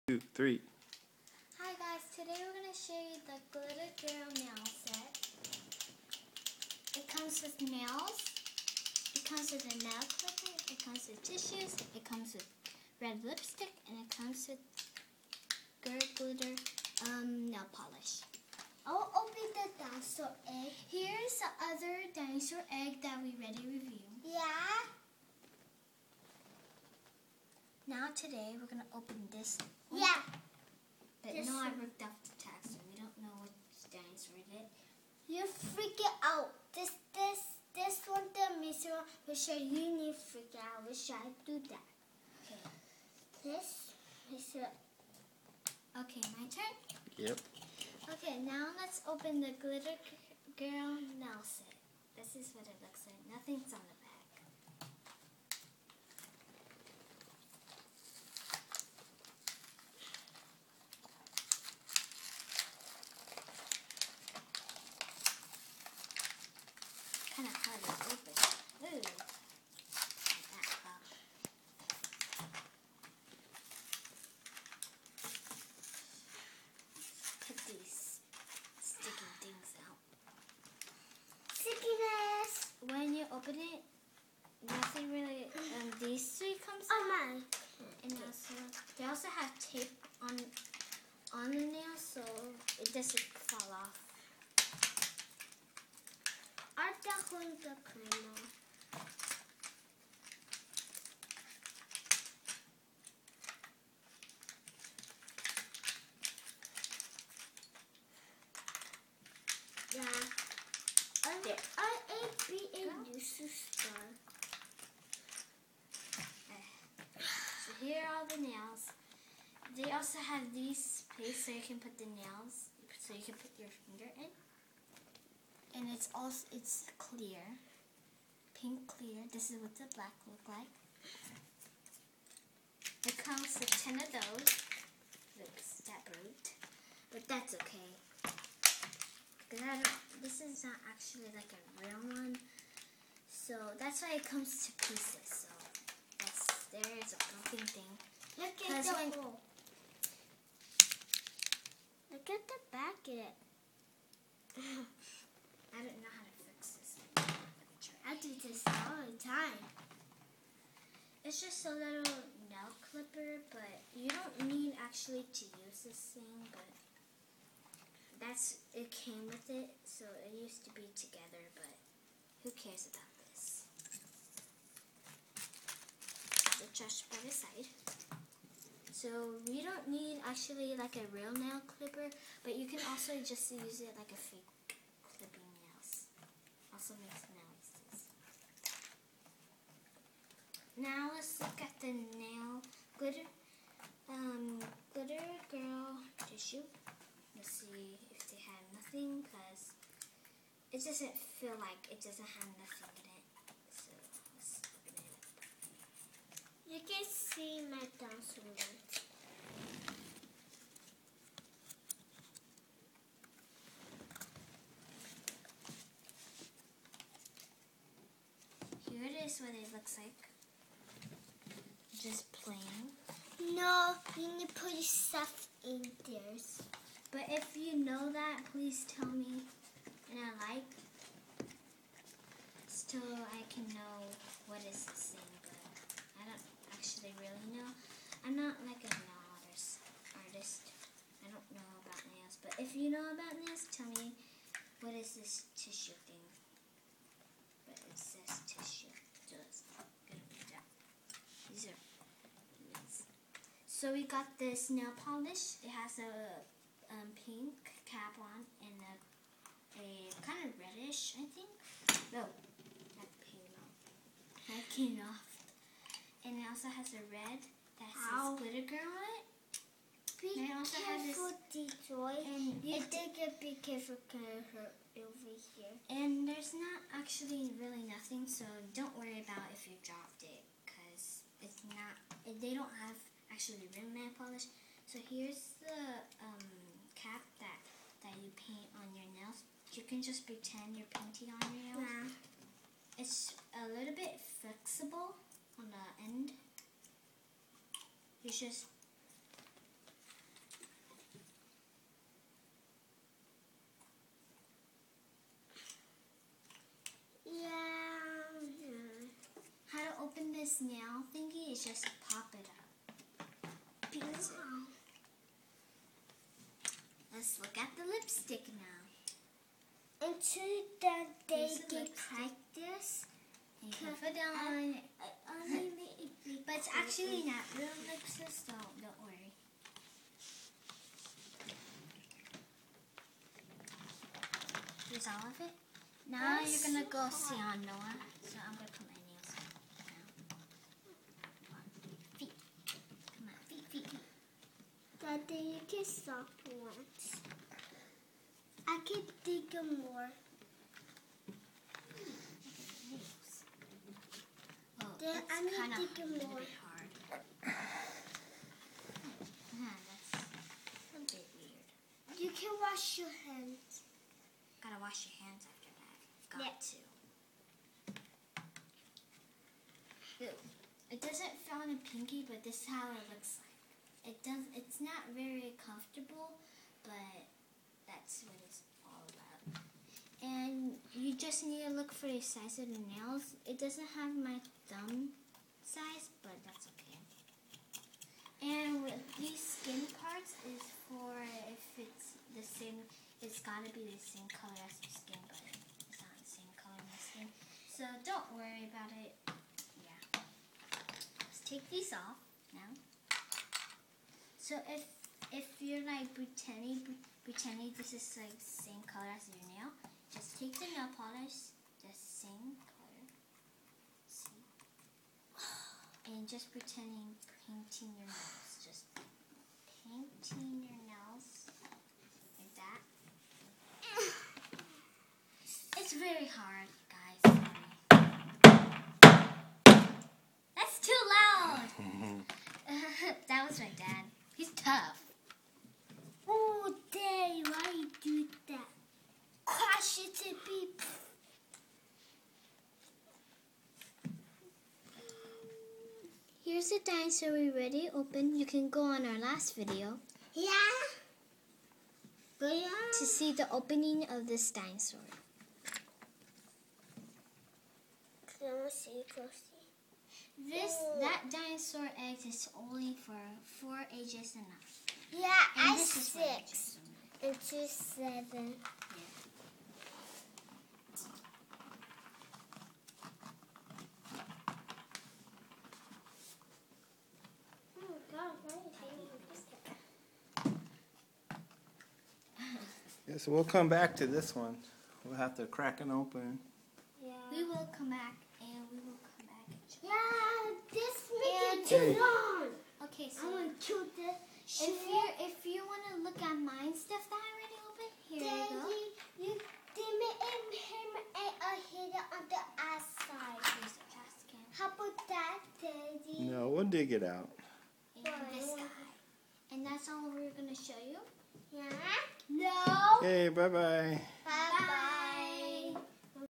Glitter Girl Nail Set and Dino World Fossil Kit
Touti and Noah showing what they got from the 99 Cent Store.